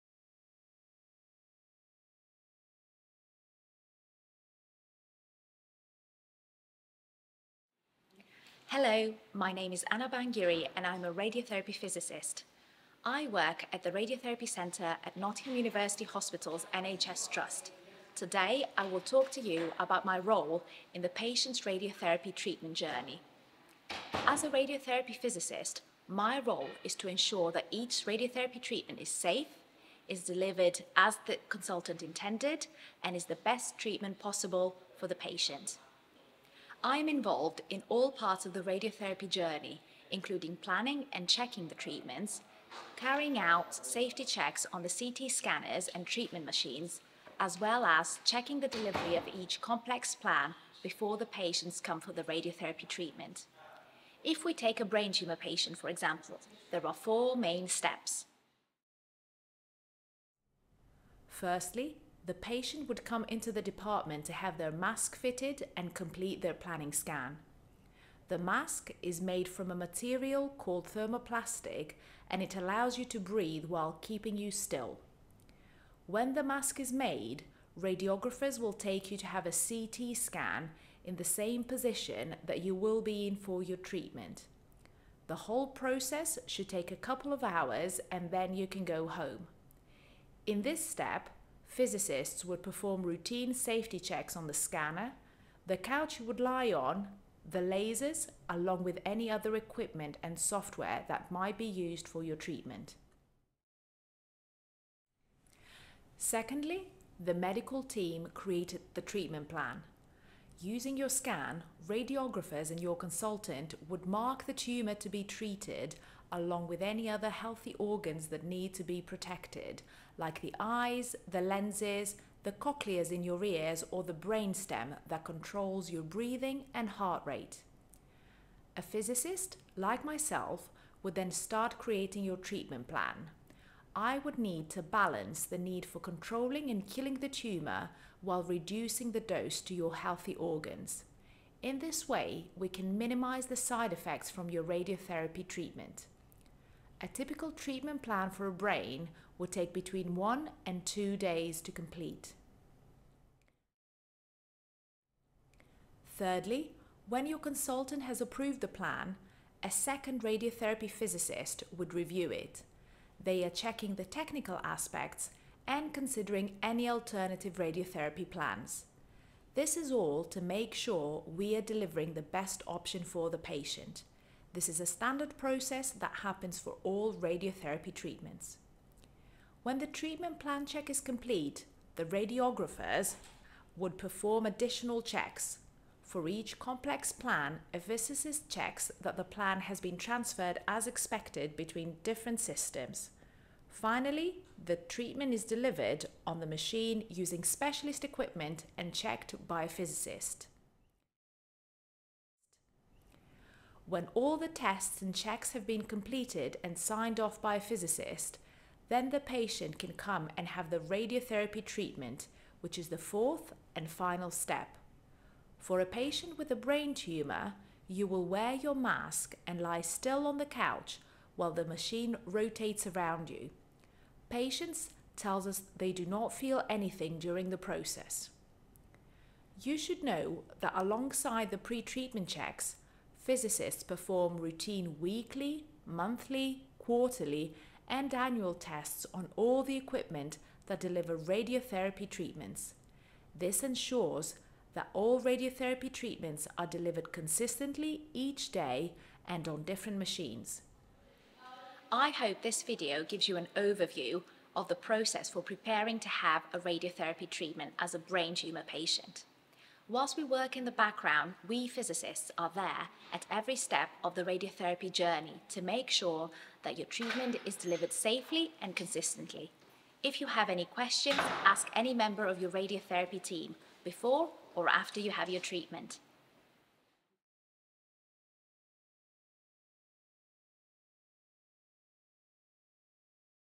Radiotherapy Physicists: Their role in the radiotherapy treatment journey
Anna Bangiri is a Radiotherapy Physicist who works at the Radiotherapy Centre at Nottingham University Hospitals NHS Trust. In this video she will talk to you about her role in the patient’s radiotherapy treatment journey.

0:00 Introduction
0:24 What is a Radiotherapy Physicist
0:58 Step 1: Mask fitting and CT scan
1:34 Step 2: Outlining and treatment plan creation
2:33 Step 3: Plan checking and tests
3:21 Step 4: Patient treatment
4:11 Conclusions